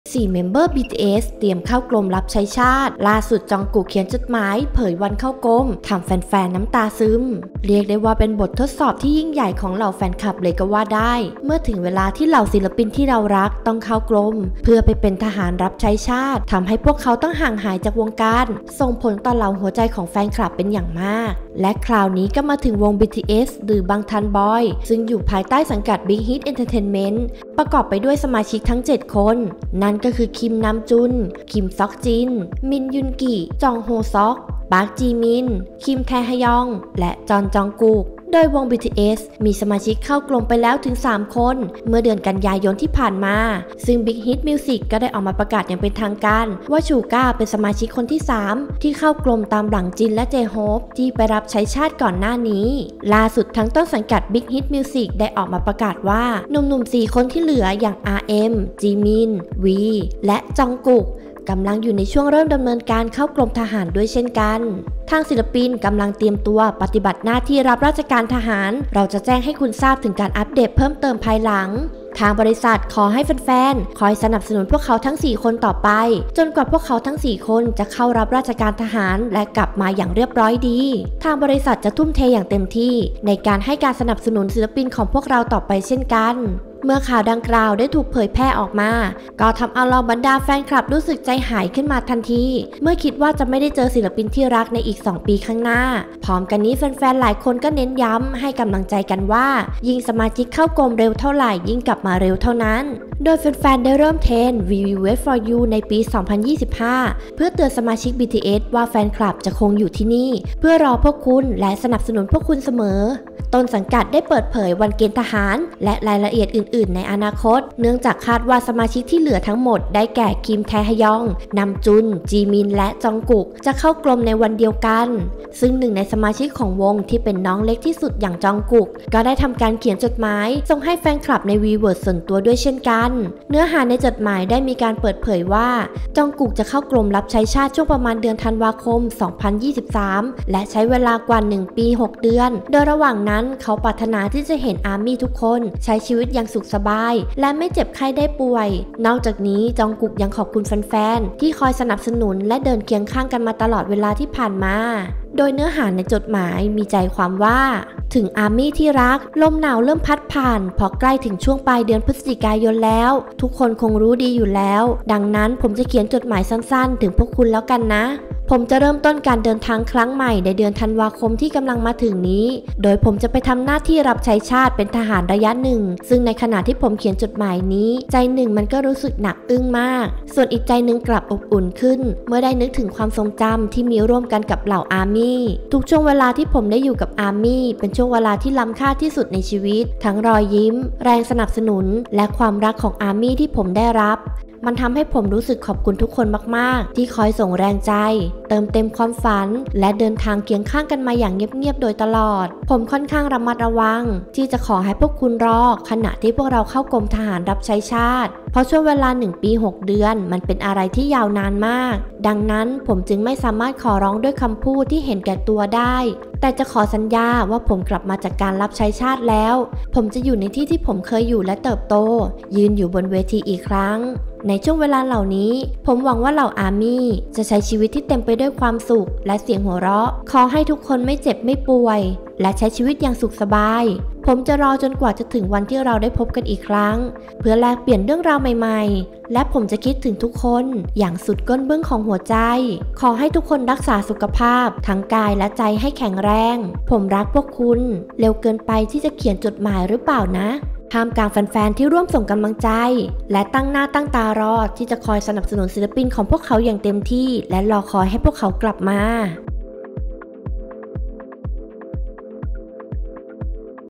อาร์มมี่ใจหาย 4 เมมเบอร์ BTS เตรียมเข้ากรมรับใช้ชาติ ล่าสุด จองกุก เขียนจดหมาย ทำแฟนๆน้ำตาซึม
อาร์มมี่ใจหาย อาร์เอ็ม-จีมิน-วี-จองกุก' 4 เมมเบอร์ BTS เตรียมเข้ากรมรับใช้ชาติ ล่าสุด จองกุก เขียนจดหมายเผยวันเข้ากรม ทำแฟนๆน้ำตาซึม